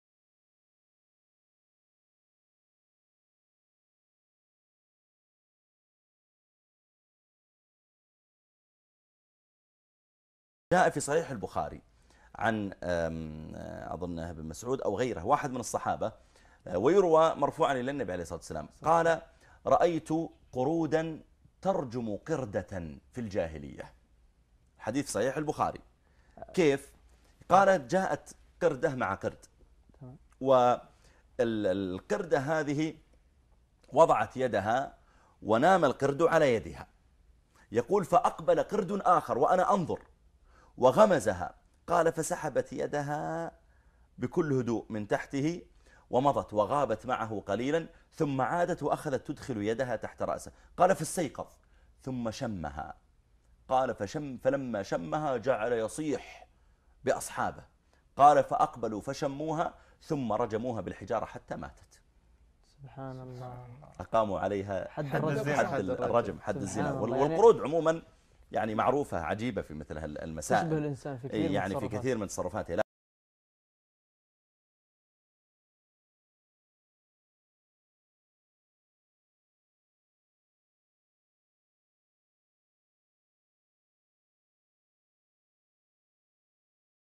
العريفي العالم الدارويني
العريفي في محاولة لمخاطبة الشباب والحديث عن الرفق بالحيوان
القرود يطبقون حد الرجم ويشبهون الإنسان في كثير من الأشياء
هذا الحديث لا يتعارض مع القرآن بشيء
لكن أي قرود كانت توجد في مكة؟